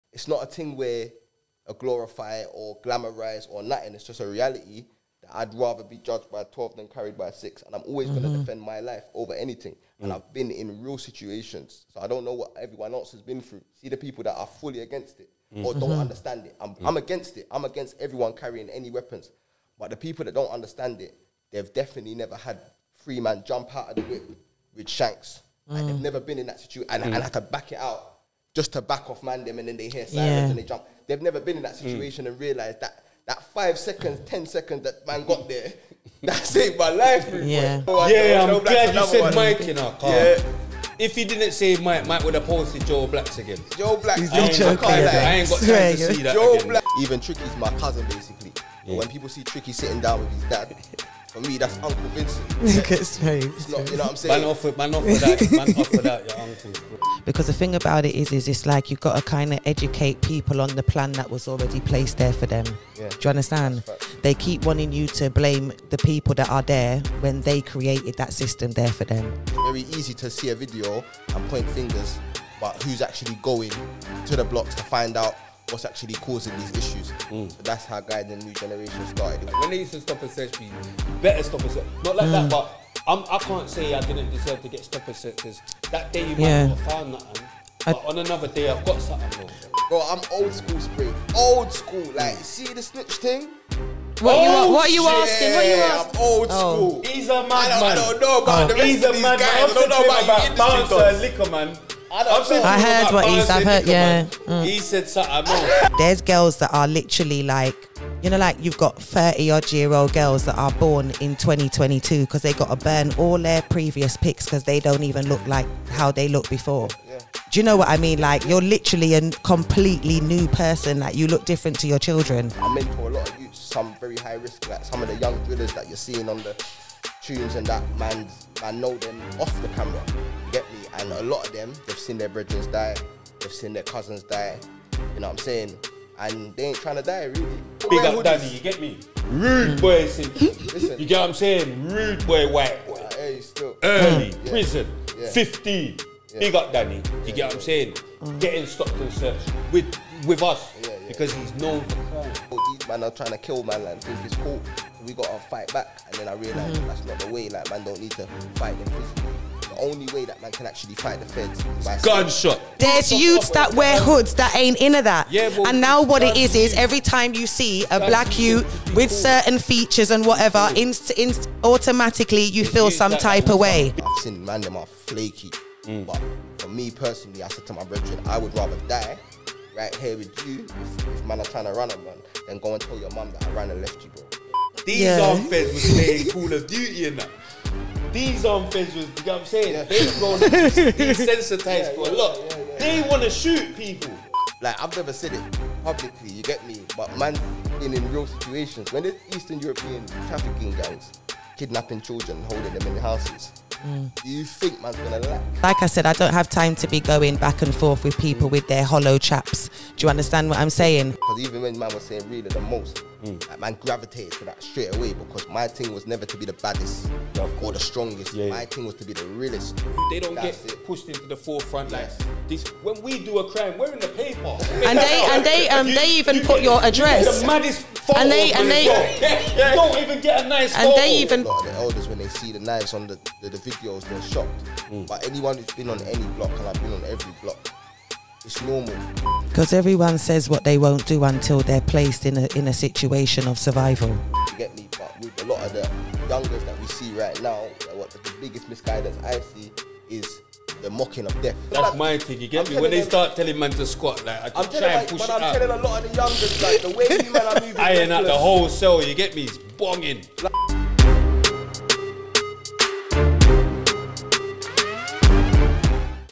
Raspect | RTM Podcast Show Season 5 Episode 1 (Trailer)
“But in the end, one needs more courage to live, than to kill himself.”

We always come back like we never left! We thought we’d start Season 5 off with one of our Kings that we feel might not get as much credit and recognition as he deserves for the work he puts in on the frontline, trying to make the streets safer for our children. Episode 1 saw us draw for the one and only Raspect to come and collect his well deserved flowers. After talking us through his upbringing and life story so far and what led him down his revolutionary path, we put Raspect through his ‘RTM Fire Round’ before getting into some ‘Current Affairs’ which included Bouncer and Likkleman’s recent reunion, Kanye West and other recent news before breaking for some ‘RTM Banter’. Then we got into the main subject which was ‘Man Up’, during which we spoke about how difficult it is for men to deal with mental health issues. The ‘RTM Dilemma’ and ‘Book Suggestion’ closed the show as usual. Raspect is such a captivating speaker that we literally could have spoken for another 6 hours! This was definitely yet another Realer Than Most conversation with lots of banter, real talk and transparency. Essential viewing to say the least. Thanks for your support, we appreciate it.

Out 30/10/22